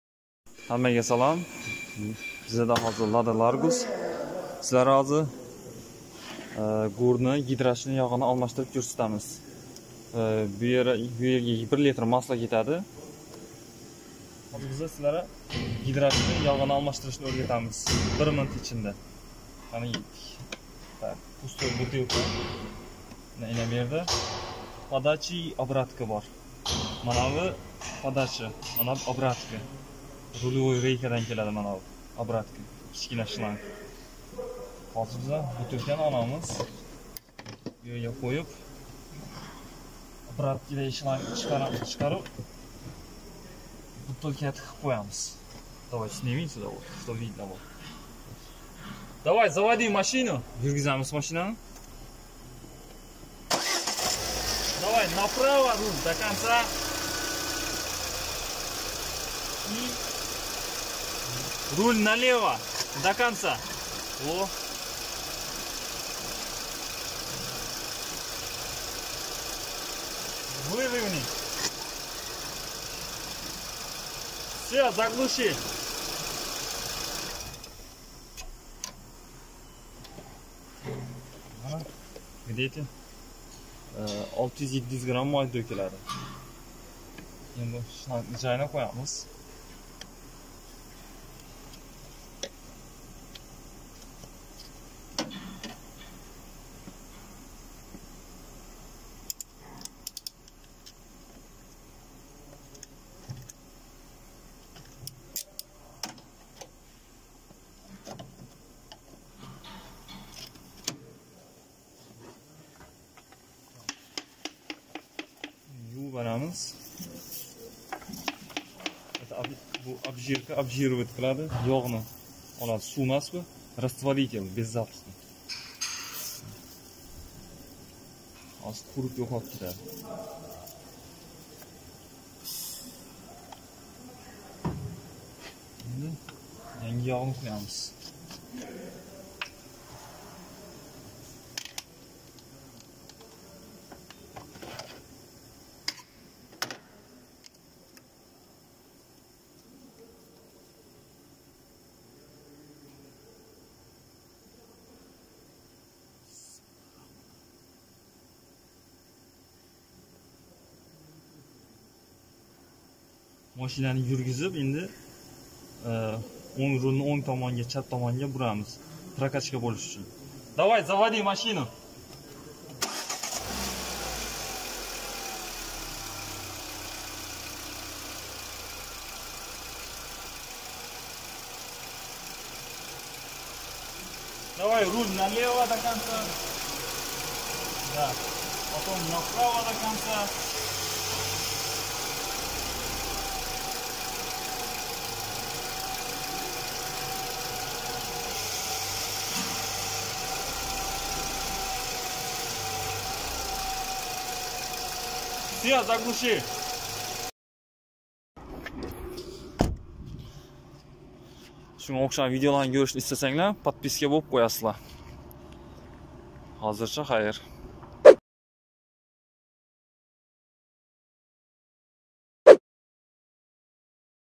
1 minutda 500 rubl ishlash
Assalom alaykum man sizlarga Lada Largusda rulni moyini almashtirishni ko'rsataman, Bu ish u-n 500-600 rubl to'lanadi, shunga o'xshagan videolarni
Ko'rishni istasanglar
Kanalga obuna bo'linglar!!!